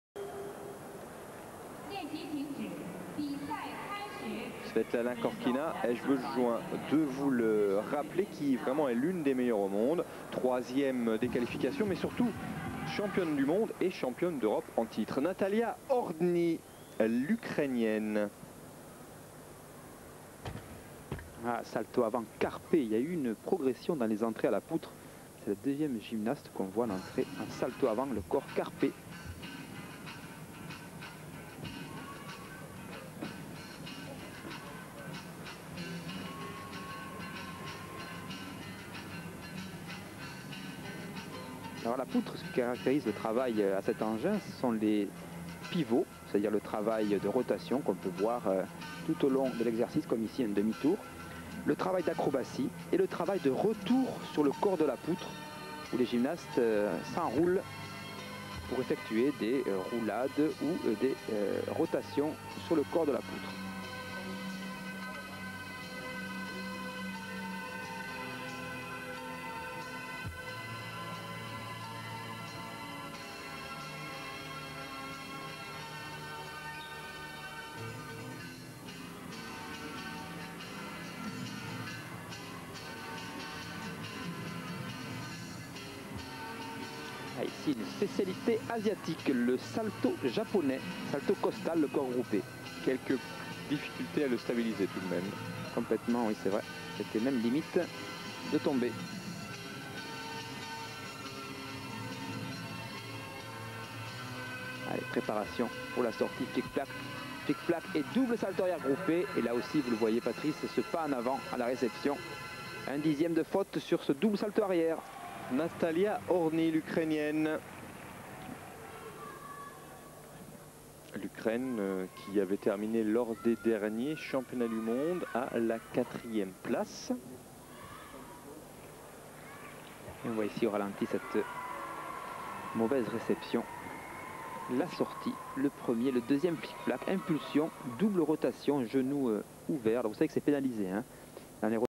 Natalia GORODNY (UKR) beam - 1999 Tianjin worlds TF
Natalia GORODNY (UKR)

'99 World championships in Tianjin - Team finals